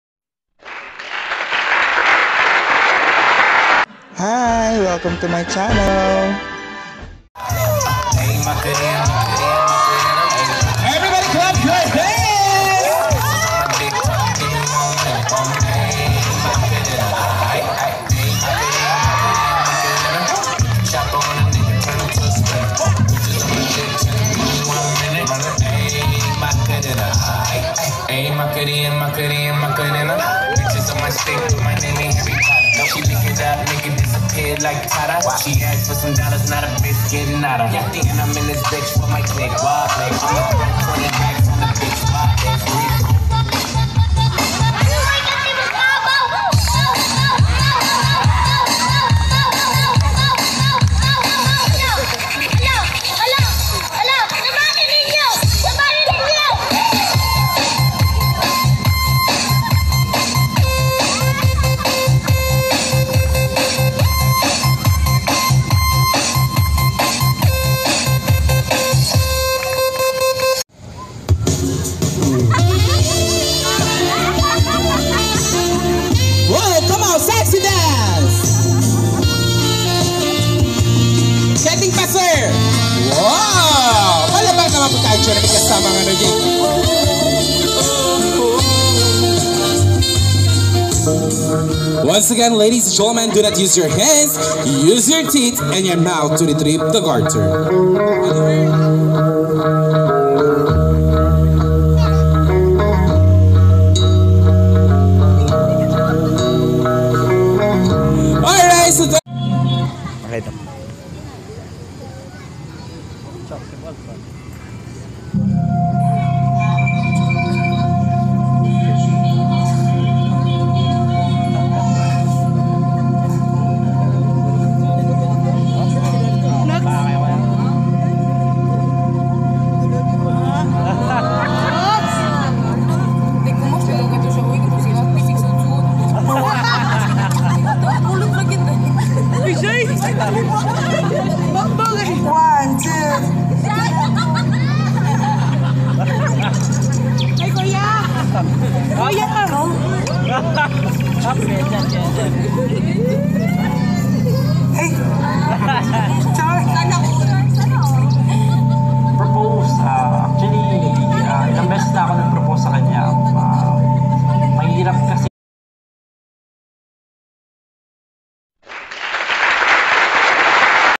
Happy moments only!
Jaypee + Sheera wedding reception moments

Full Styling c/o Mind Creation Events Styling

Catered by Bright Events Catering Services and Bright Events Coordination